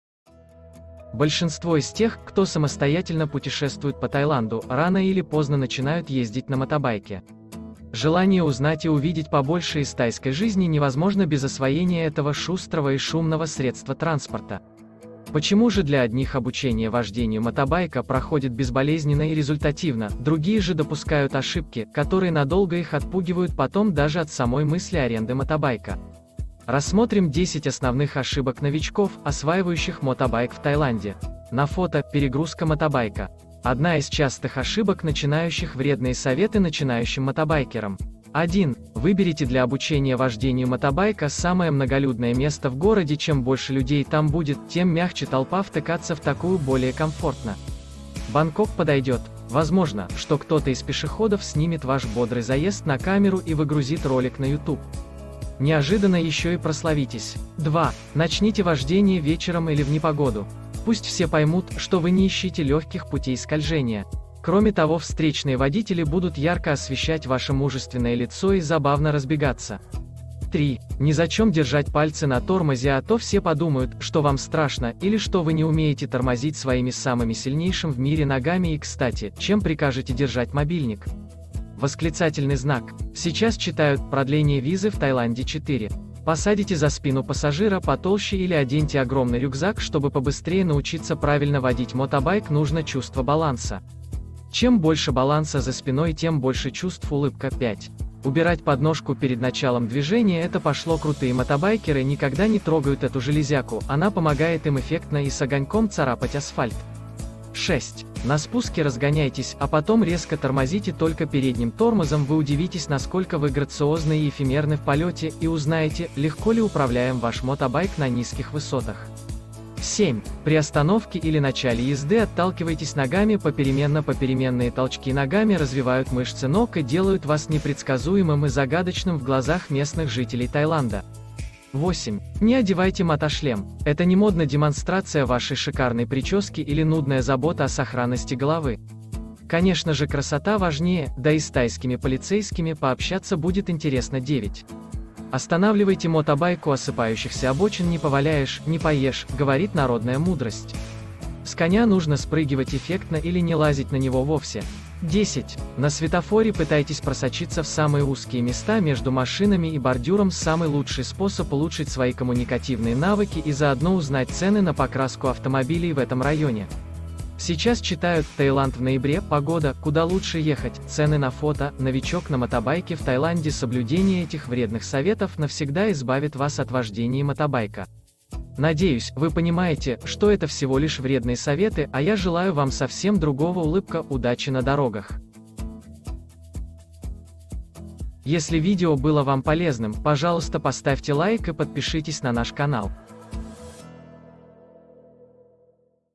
Мотобайк в Тайланде: 10 вредных советов начинающим
Почему же для многих обучение вождению мотобайка проходит безболезненно и результативно, другие же допускают ошибки, которые ..